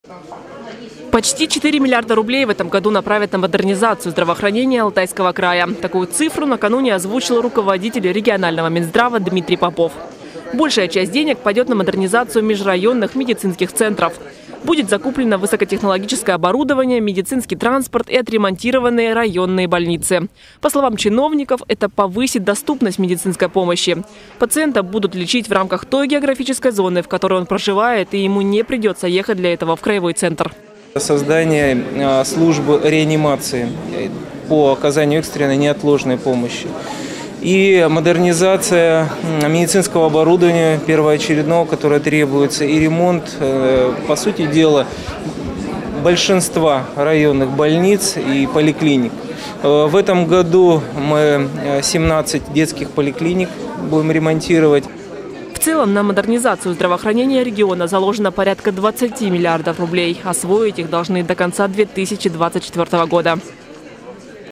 На модернизацию здравоохранения Алтайского края направят свыше 20 миллиардов рублей
Почти четыре миллиарда рублей в этом году направят на модернизацию здравоохранения Алтайского края. Такую цифру накануне озвучил руководитель регионального минздрава Дмитрий Попов.